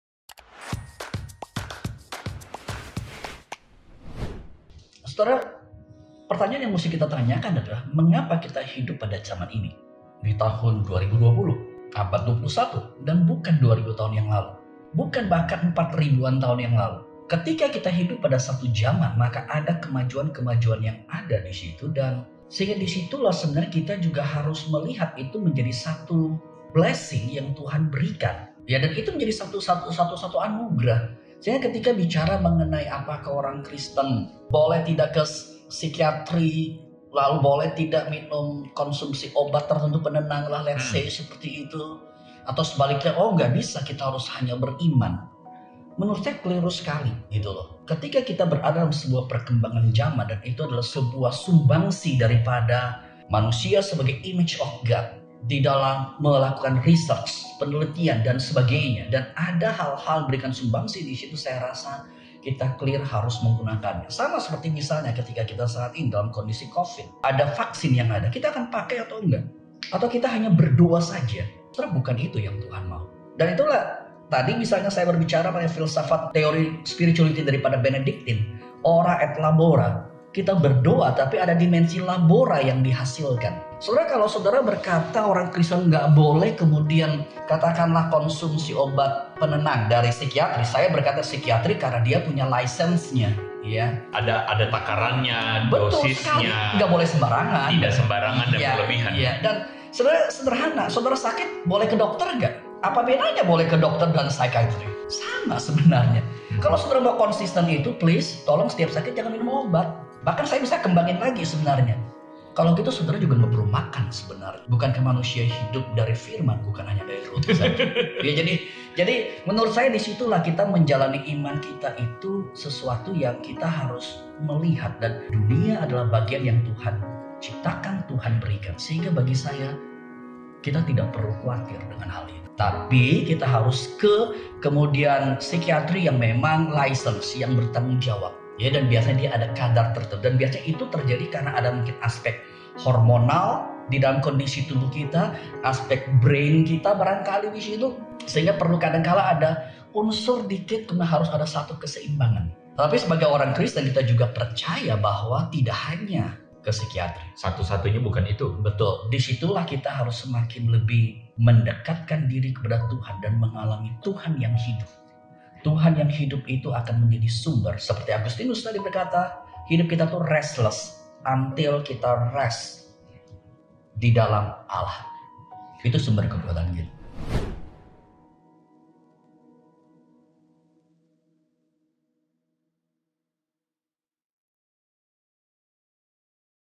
Psikiatri VS Kristen? | HelloGod Teaching | with Pdt. Tommy Elim
HelloGod bersama dengan Tommy Elim membicarakan mengenai kekristenan dan psikiatri. Apakah orang kristen boleh ke psikiatri atau mengkonsumsi obat-obat tertentu?

Kita hidup di zaman ini adalah suatu anugrah dari Tuhan. Pada satu zaman ke zaman, pasti ada kemajuan ilmu yang berkembang. Dan hal itu juga adalah bagian yang Tuhan ciptakan.

Nonton series lainnya di app HelloGod. Ayo download aplikasinya.